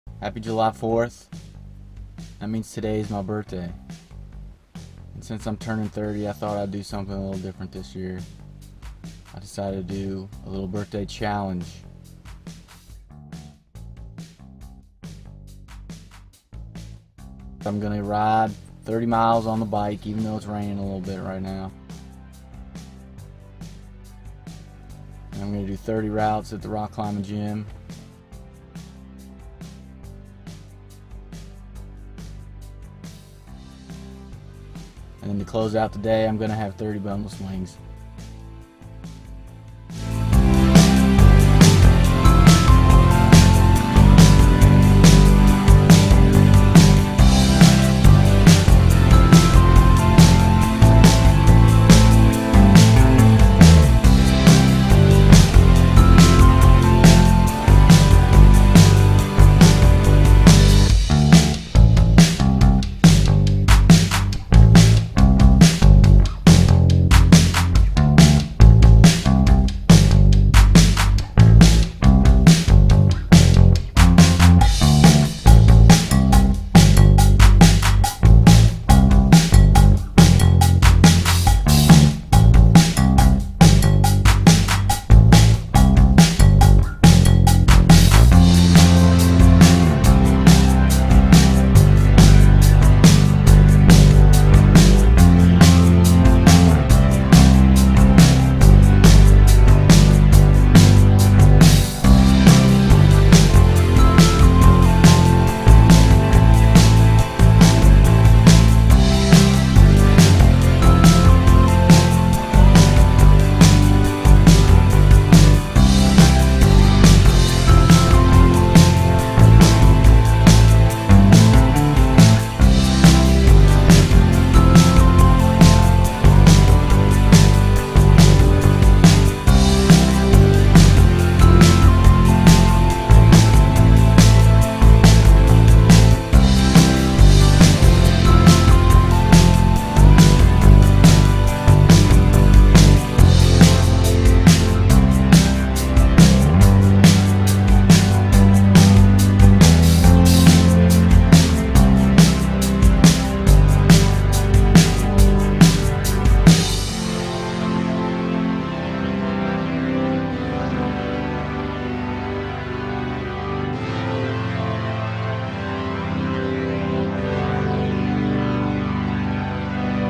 Jeffrey's 30th Birthday Challenge 2016
This is my first annual birthday challenge for my 30th birthday.  I decided for my challenge to ride 30 miles on the bike (even though it rained), climb 30 bouldering v-points, and climb 30 top rope routes at the gym.  I finished it off with a celebratory dinner of 30 boneless wings.  It was a true challenge but it was a blast.  Let me know what you think of my video and what you think I should do next year.